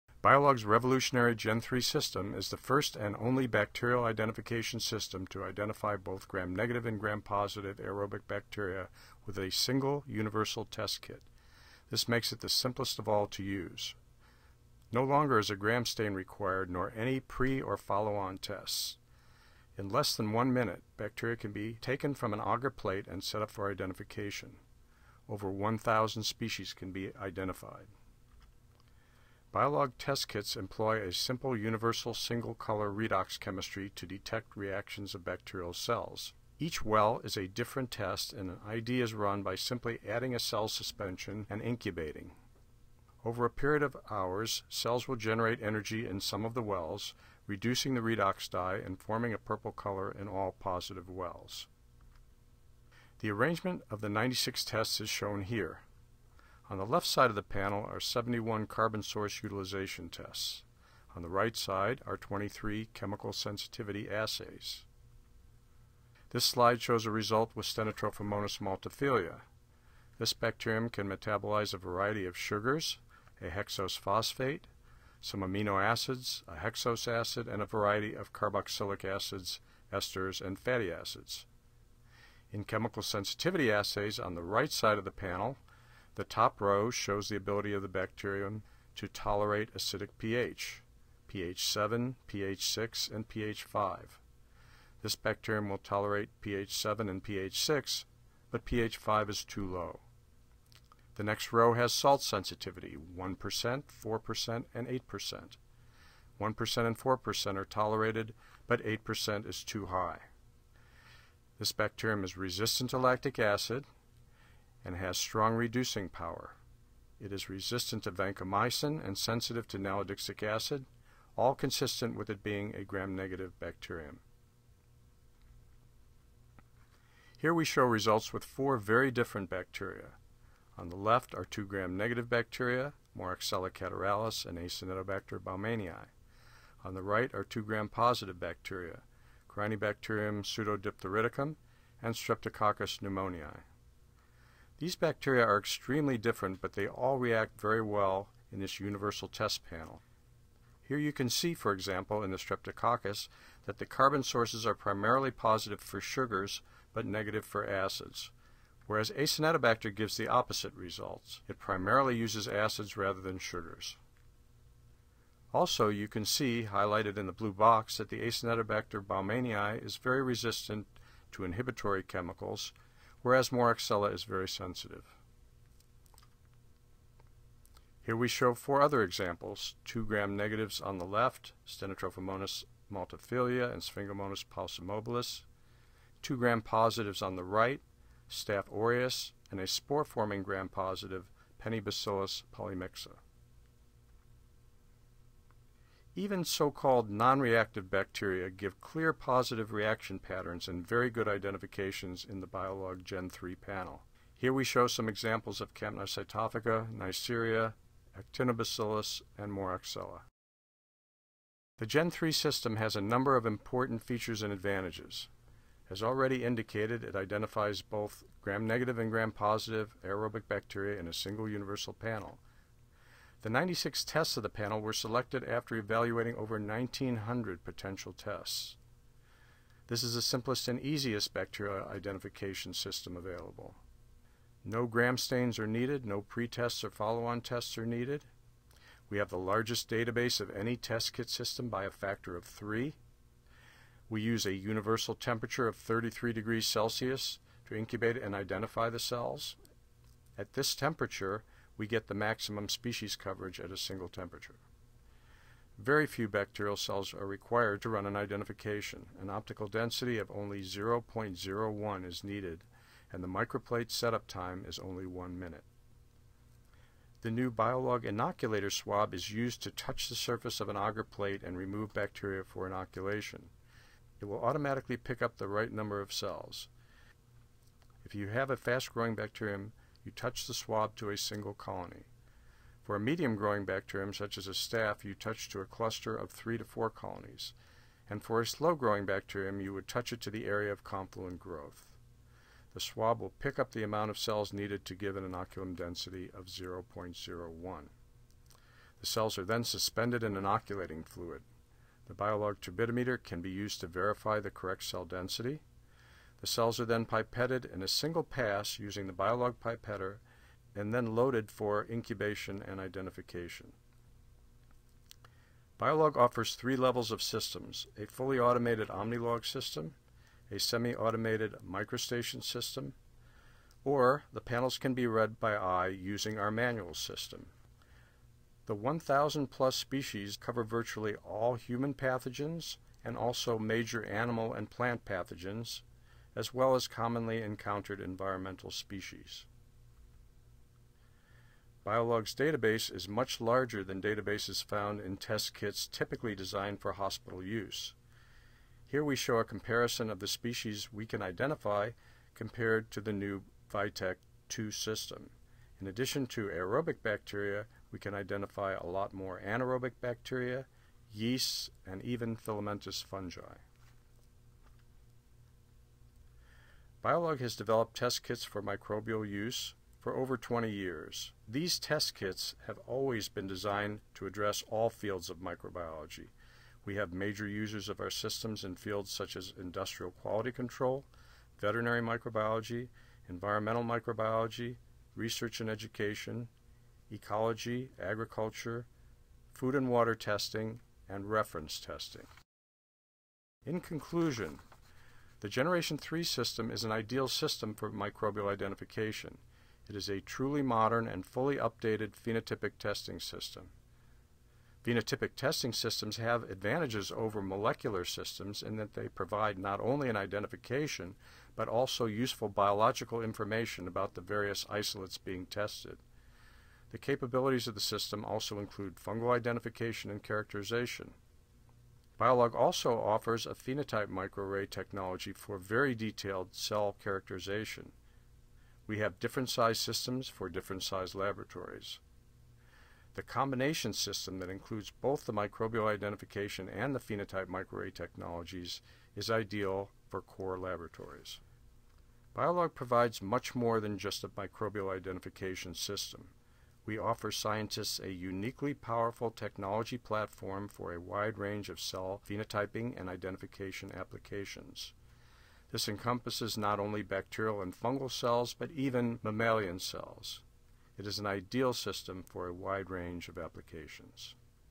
Microbial Identification using Biolog's GEN III Technology.mov
Microbial Identification that is fast, simple and accurate.  Biolog has system sizes to suit any lab.